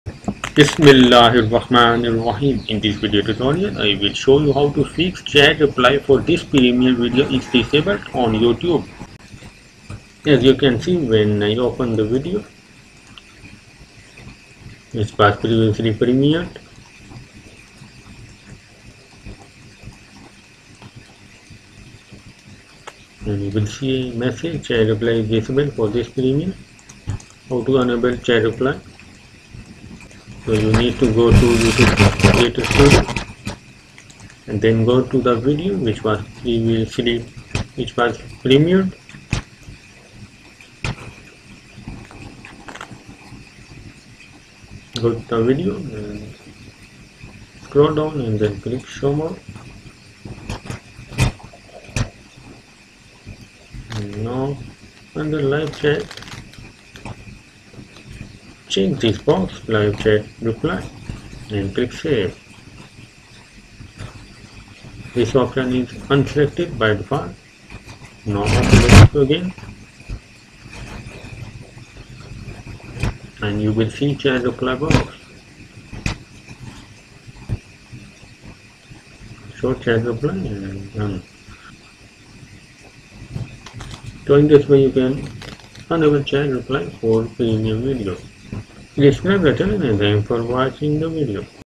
How To Fix Chat Replay Is Disabled For This Premiere On YouTube (2023)
You can enable / turn on chat replay for your video premiered earlier. By default, live chat replay box is disabled / turned off after the premiere has been ended. You can enable and disable live chat replay at any time.